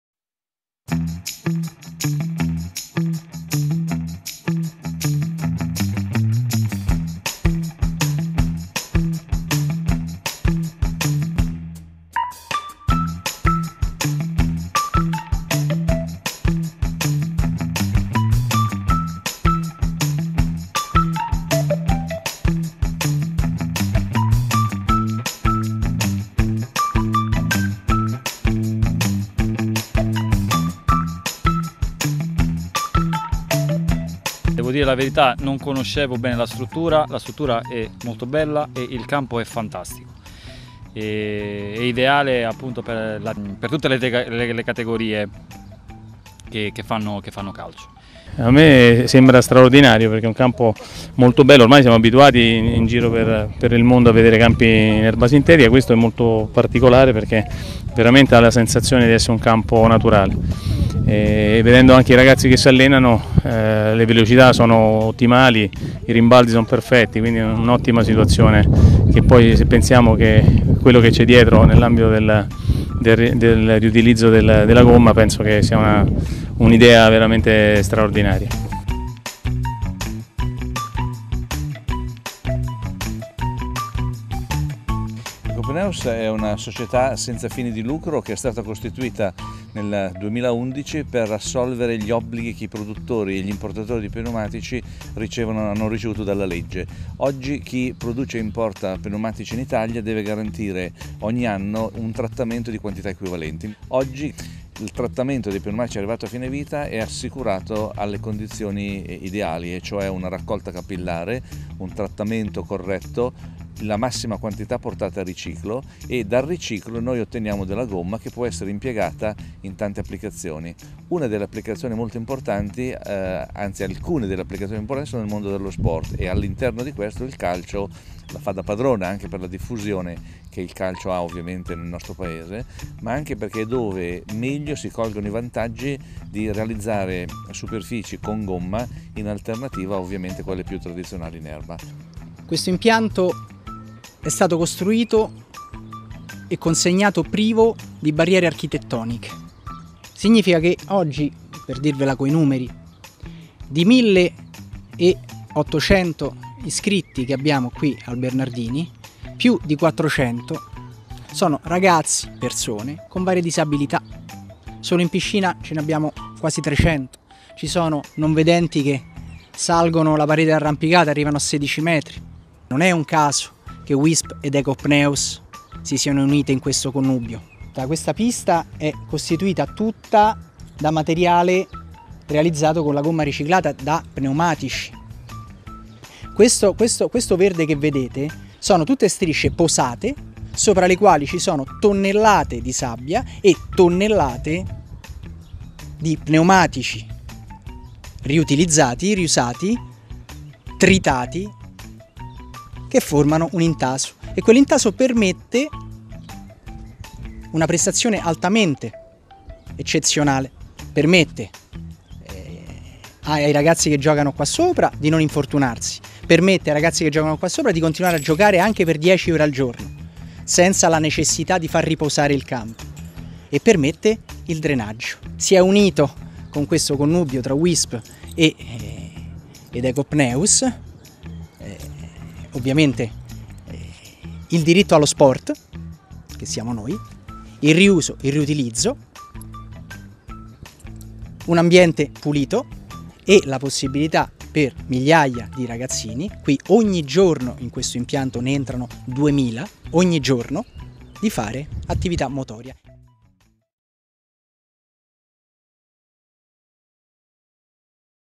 Uisp ed Ecopneus all'impianto Fulvio Bernardini per i numeri1 del calcio
Sabato 9 giugno a Roma uno stage formativo per giovani portieri. Le interviste a Marco Garofalo preparatore dei portieri del settore giovanile SS Lazio, Claudio Filippi, allenatore dei portieri Juventus FC, Giovanni Corbetta direttore generale Ecopneus, Alessandro Bachi direttore Impianto Fulvio Bernardini